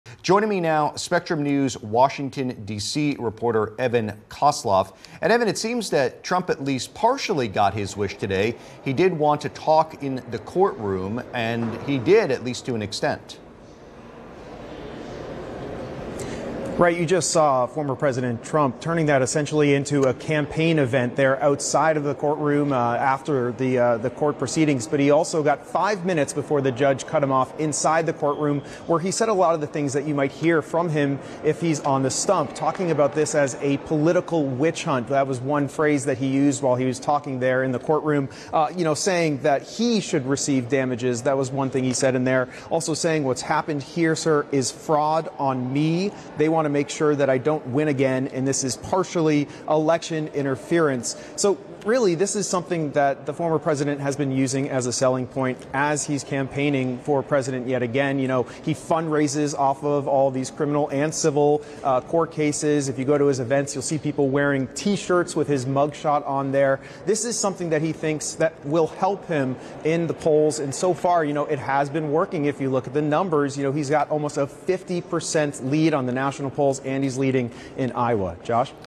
Live Analysis of Closing Arguments in Trump's New. York Civil Fraud Case
Evan Koslof is an 8x Emmy award-winning reporter, working as a National Political Correspondent for Spectrum News stations across 91 markets and 36 states.